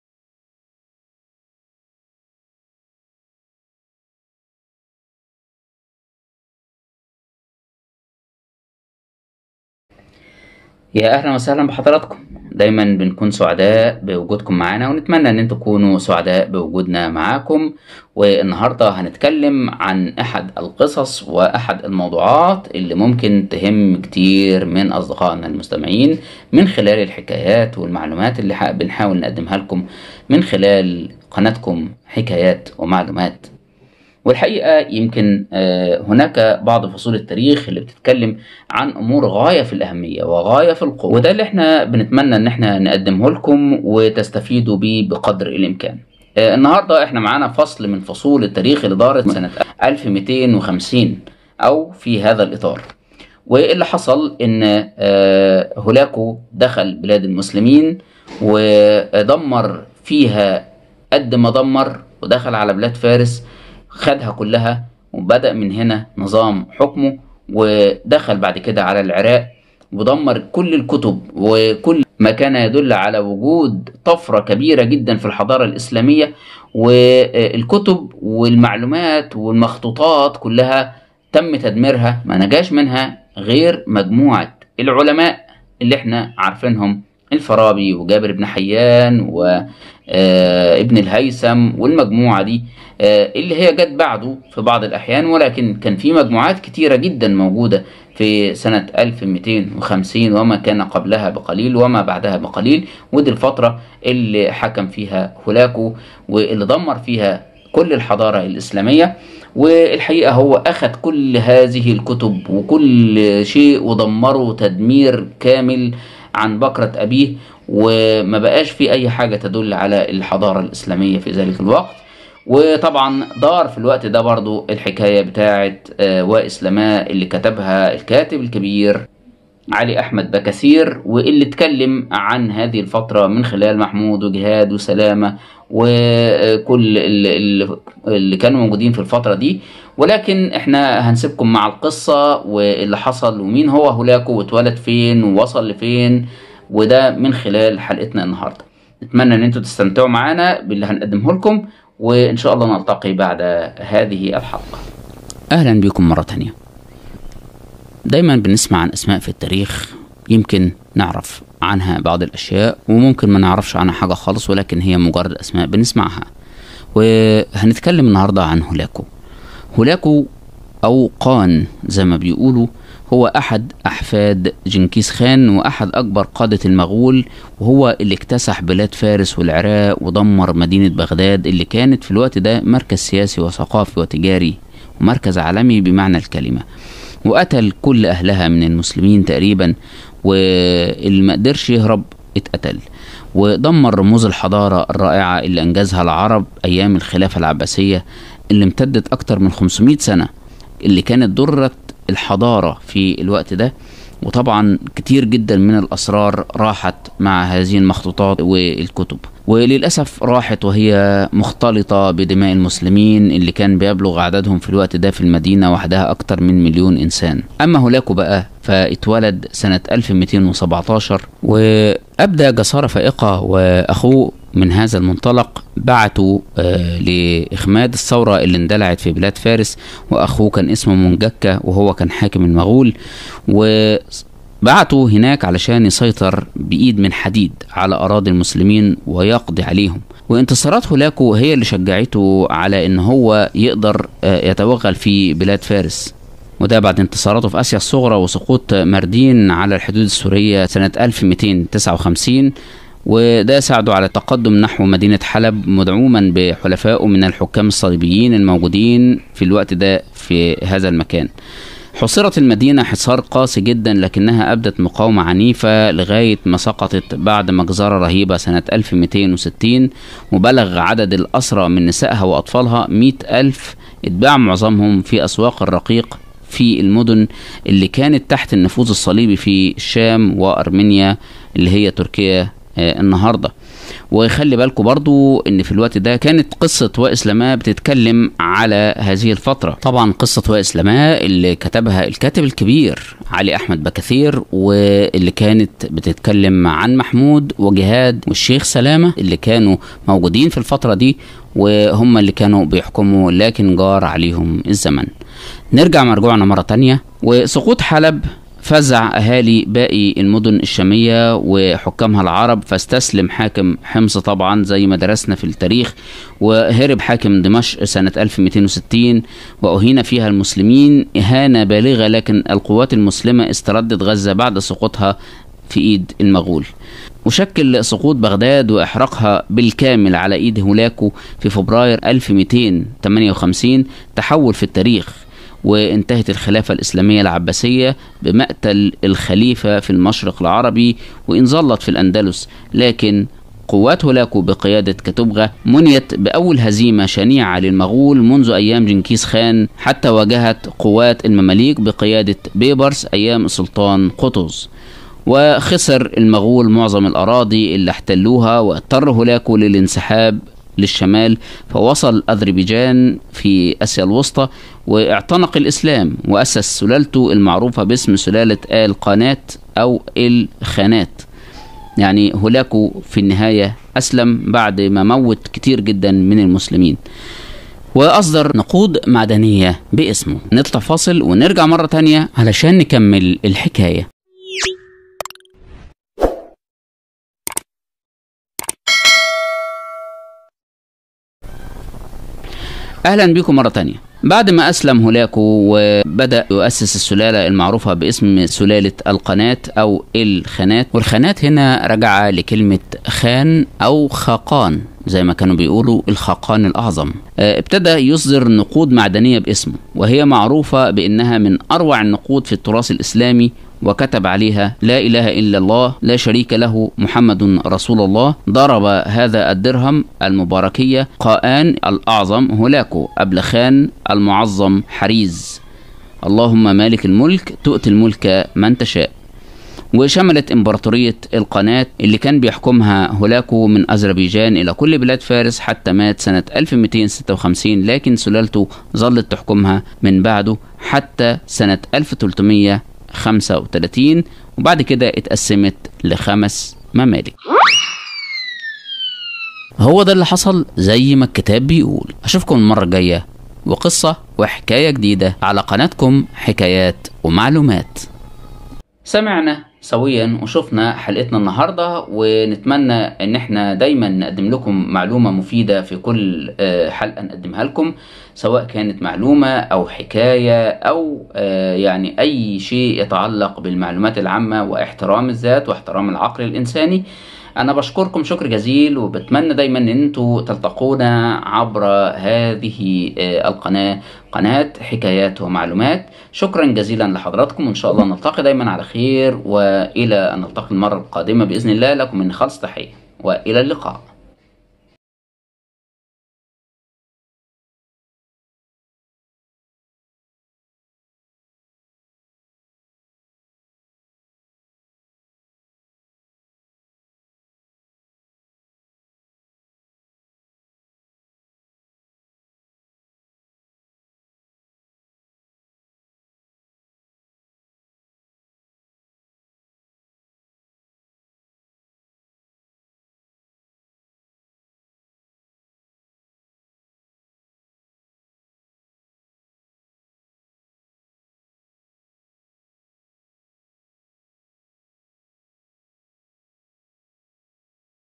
عندما اسلم هولاكو
حكاية هولاكو وكيف دخل الي بلاد الاسلام وعلاقه محمود و جهاد وسلامه به

الخاقان الاعظم هولاكو